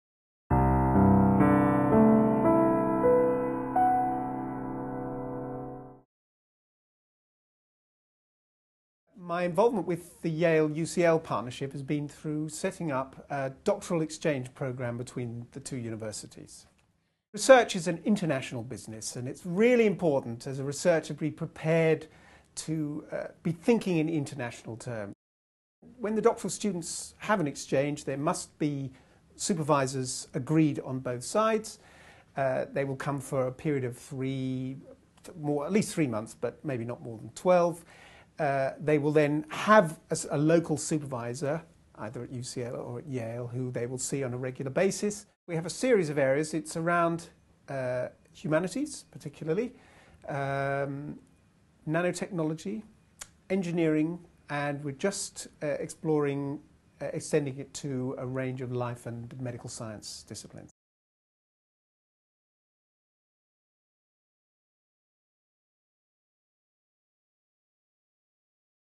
Yale UCL: Coordinated PhD Program
David Bogle, Head of the UCL Graduate School, provides an overview of the Yale UCL Coordinated PhD Program.  This video wiill be of interest to prospective students.